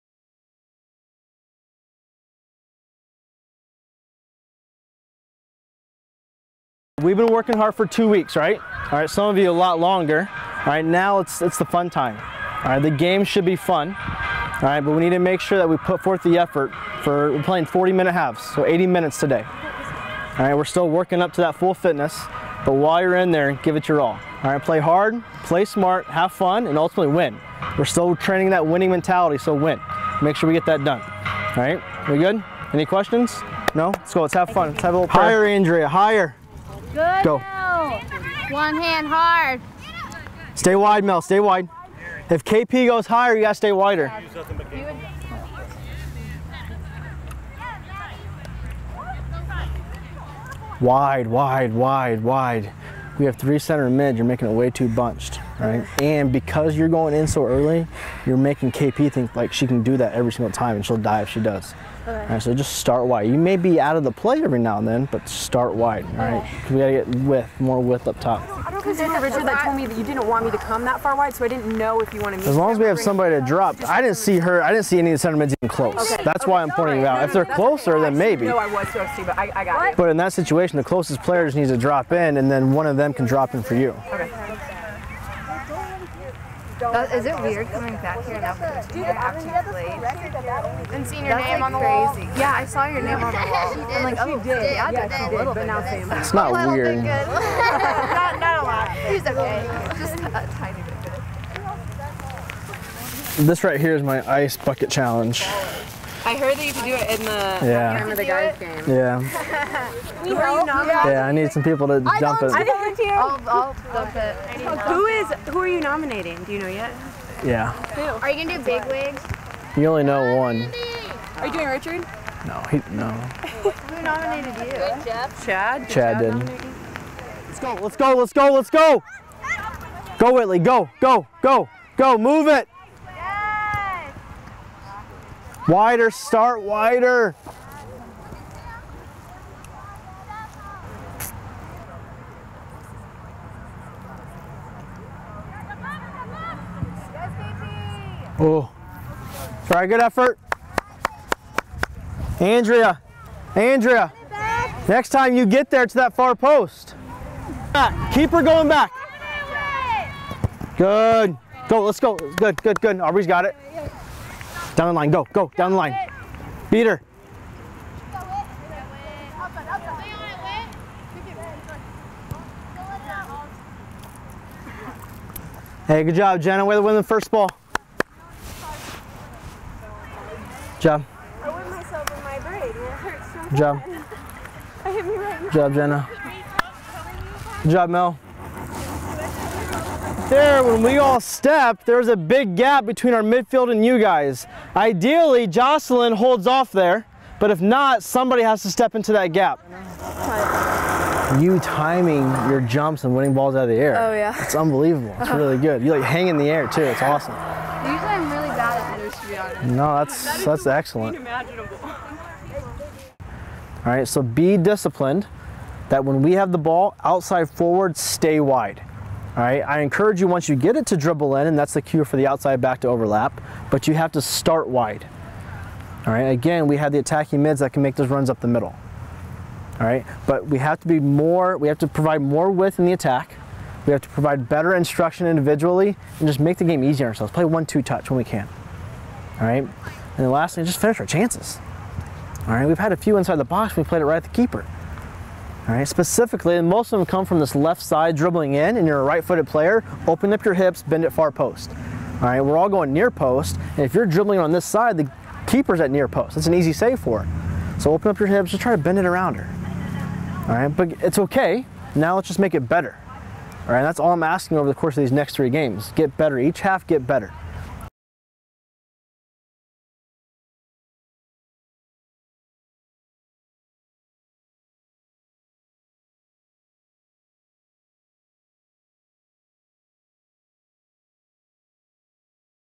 Mic'd Up - Gary Yohe
Throughout the 2014-15 season, MUKNIGHTS.com will ask coaches to be Mic'd Up during practice or a game. Head women's soccer coach Gary Yohe is featured in this session of Mic'd Up.